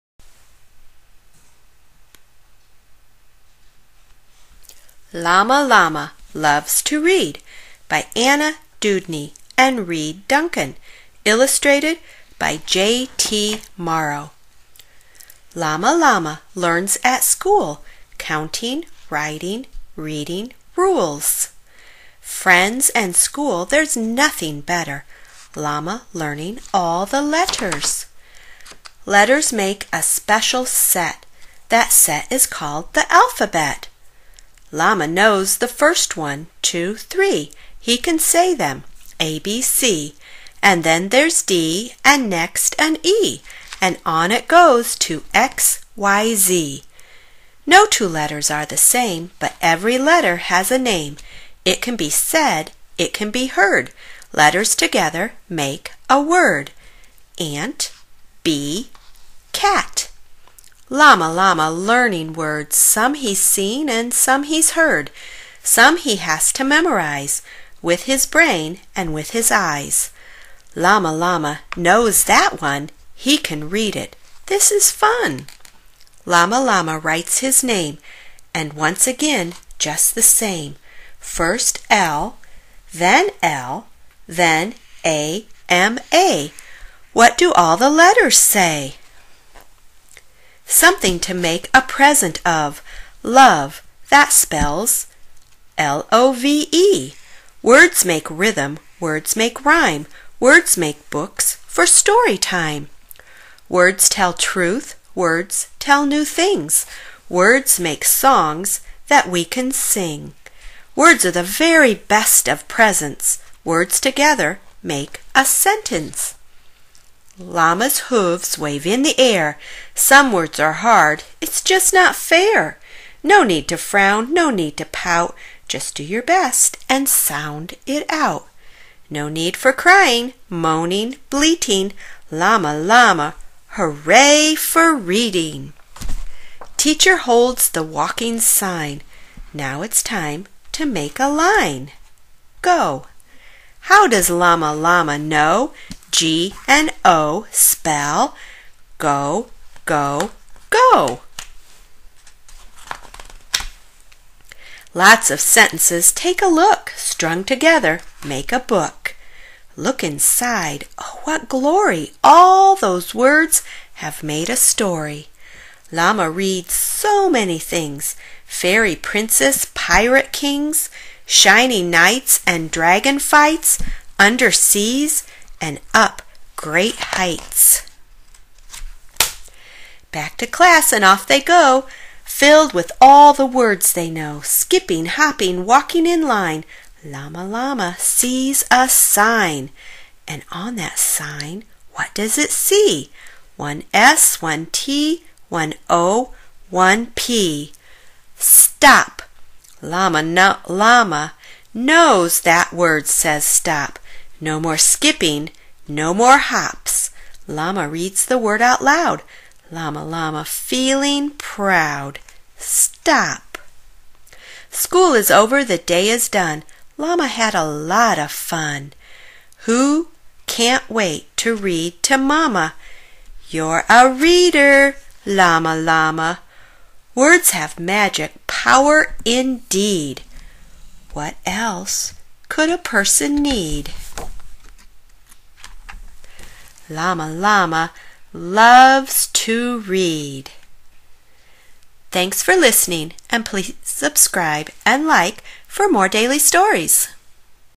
LlAMA LlAMA LOVES TO READ
Another fun book by Anna Dewdney, Reed Duncan and JT Morrow! Learning to read can be fun, so come along, join in on the fun and learn with Llama Llama.